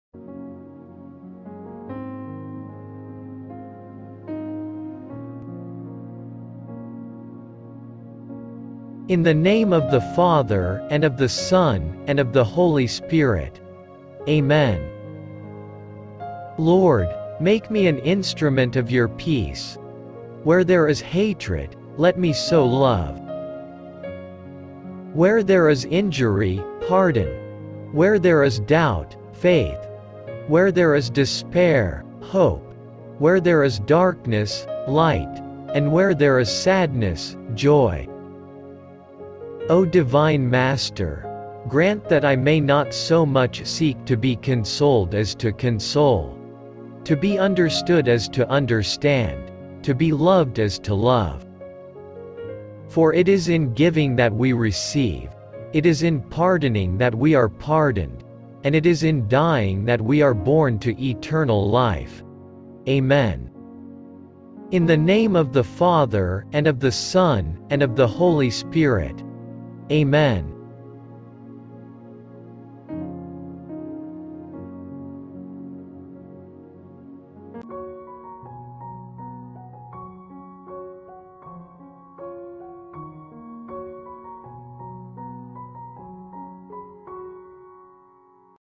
Prayer of Saint Francis of Assisi | Patron of the Poor | Feast, October 4| InJesusName
Born in Italy circa 1181, Saint Francis of Assisi was renowned for drinking and partying in his youth. After fighting in a battle between Assisi and Perugia, Francis was captured and imprisoned for ransom. He spent nearly a year in prison — awaiting his father's payment — and, according to legend, began receiving visions from God. After his release from prison, Francis heard the voice of Christ, who told him to repair the Christian Church and live a life of poverty. Consequently, he abandoned his life of luxury and became a devotee of the faith, his reputation spreading all over the Christian world.

Later in life, Francis reportedly received a vision that left him with the stigmata of Christ — marks resembling the wounds Jesus Christ suffered when he was crucified — making Francis the first person to receive such holy wounds. He was canonized as a saint on July 16, 1228. During his life he also developed a deep love of nature and animals and is known as the patron saint of the environment and animals; his life and words have had a lasting resonance with millions of followers across the globe. Each October, many animals the world over are blessed on his feast day.

InJesusName